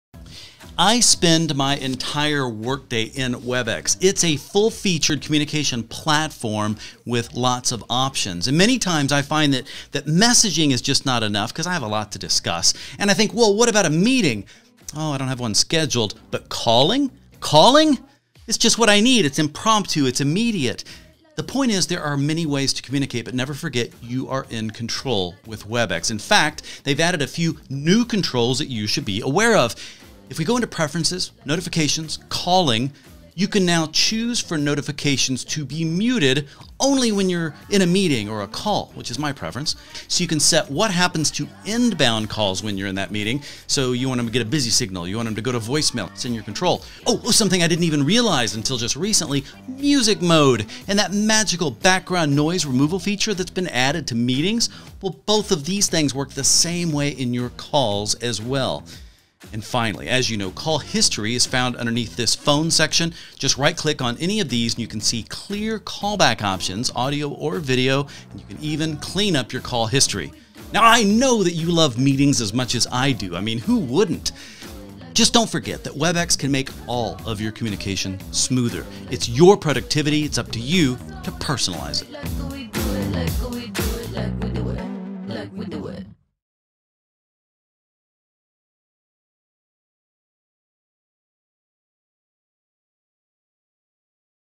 Personalize your calling experience | Webex Calling
Explore new calling preferences and controls to optimize your experience—set call notifications when you're in a meeting or on a call, background noise removal and music mode, and manage your call history. Improve your productivity in Webex by personalizing your calling experience.